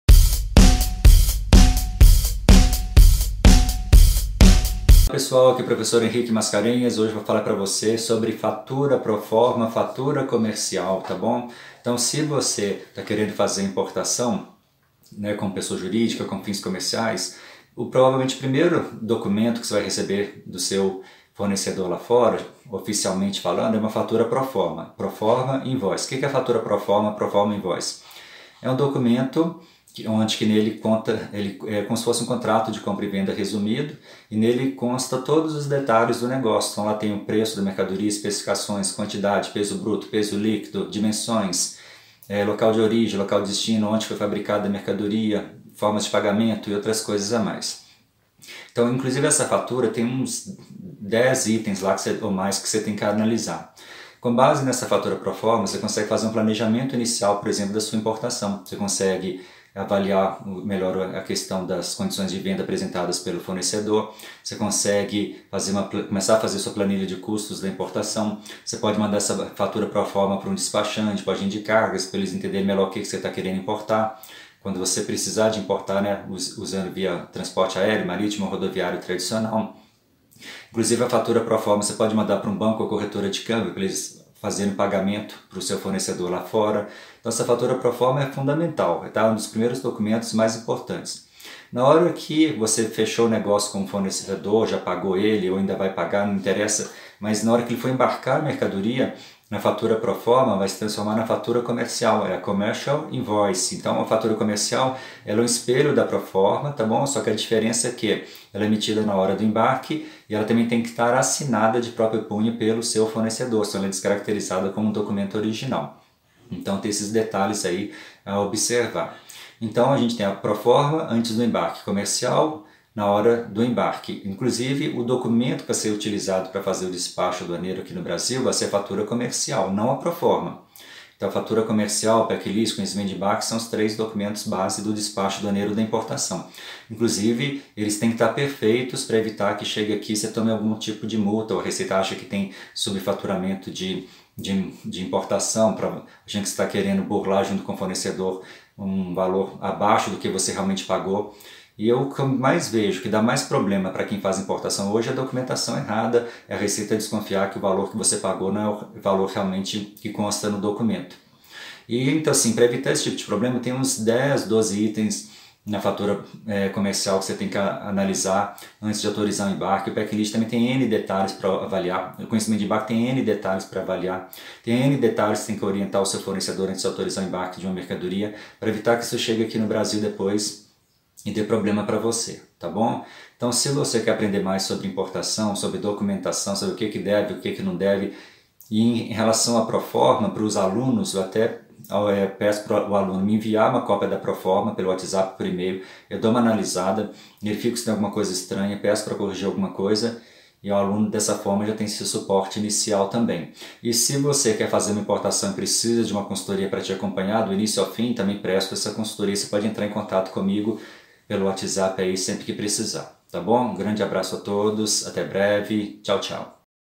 Fatura comercial e documentos na importação
Você sabe quais são as diferenças entre a fatura comercial e a fatura proforma que são usadas na importação e exportação?

Você sabe que a fatura proforma é usada antes do embarque e a fatura comercial no momento do embarque?

Durante o processo de fiscalização aduaneira a fatura comercial também deverá ser utilizada.

Se você deseja fazer importação e exportação como pessoa jurídica e finalidade comercial, assista este vídeo.